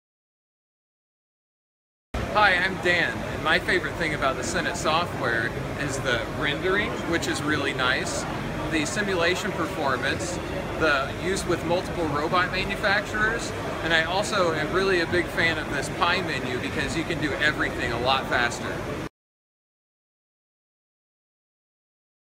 AUTOMATICA 2016 - My favorite thing about CENIT's FASTSUITE...
We present our high-performance FASTSUITE Edition 2 Digital Factory software at the AUTOMATICA 2016 trade fair.